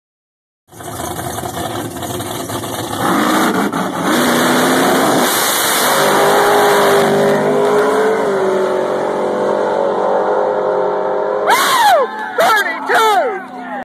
Shawn sets the new record in Mod Street at TVRP
Shawn Ellington sets the mod street record in his 1955 chevy. 5.32 @ 134mph